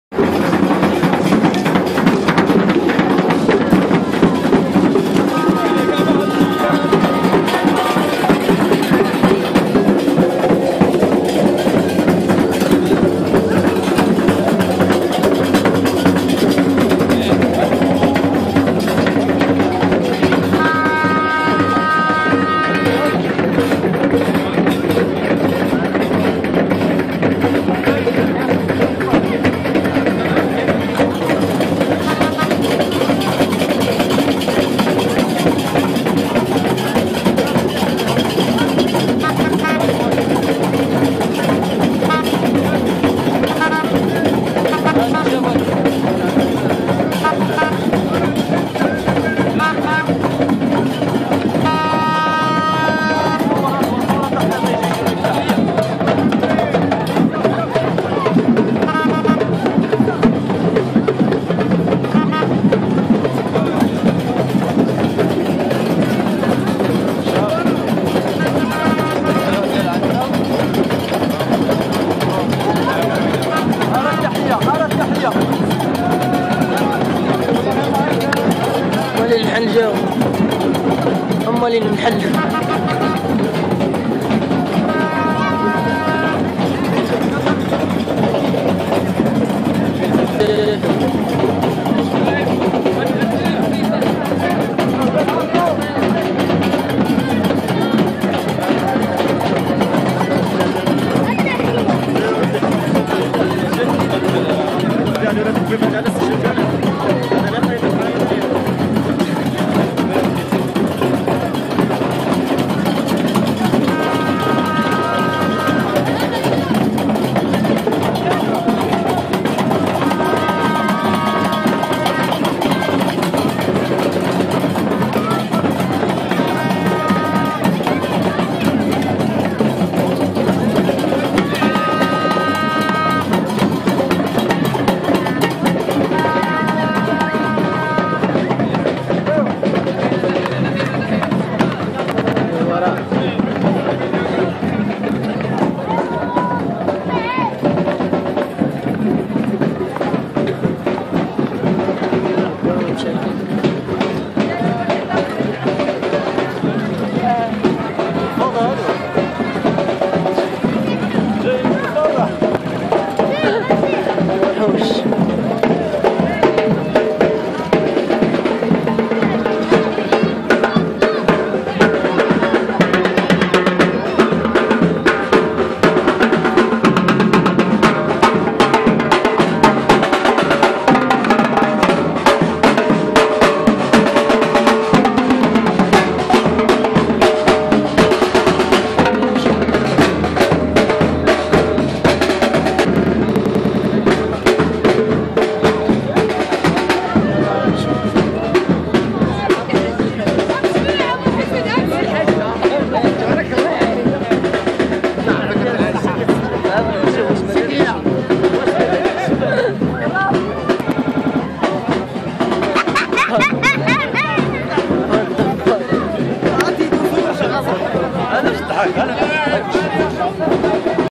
Mckankon2017 ( wald howara crazy festival)in my contry morocco a city ouled teima
Howara boujloud 3alam ibda3 lmajanin